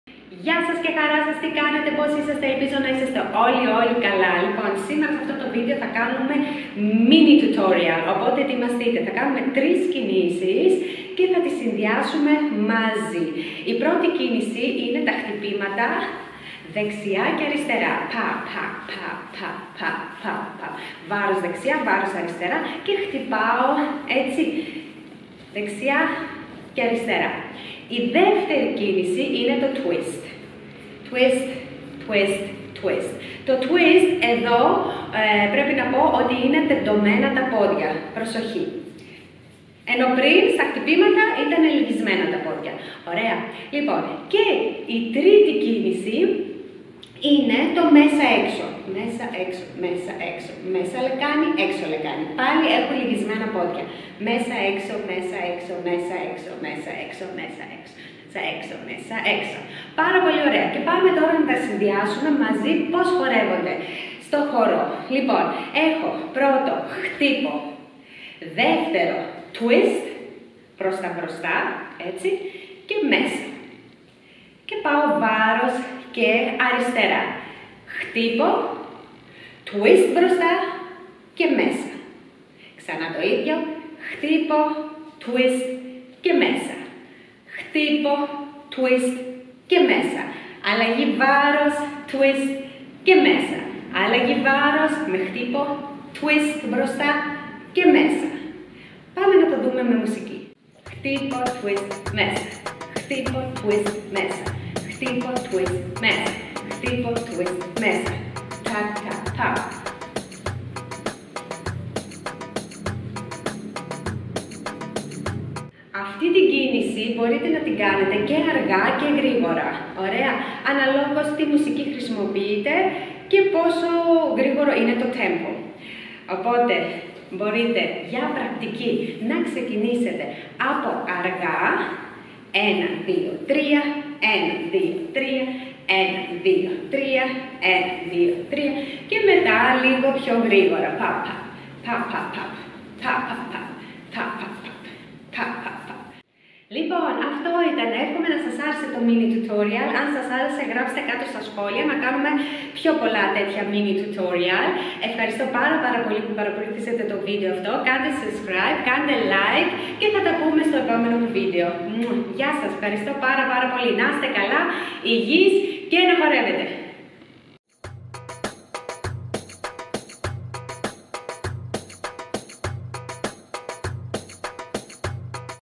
Bellydance Mini Tutorial | Μάθημα χορού Οριενταλ για αρχάριους (Hits, Twist, inside)
📺 Γράψε μου στα σχόλια αν τα κατάφερες να το κάνεις και αν σου άρεσε το Mini Tutorial!!

Lia Verra • Oriental dance Teacher - Choreographer from Greece 🇬🇷

سلسلة الرقص الشرقي للمبتدئين -ازاحة الورك

Bellydance Mini Tutorial | Μάθημα χορού Οριενταλ για αρχάριους درس رقص شرقي

Keep dancing !! ♡

Thank you Dance Spirit for the Space!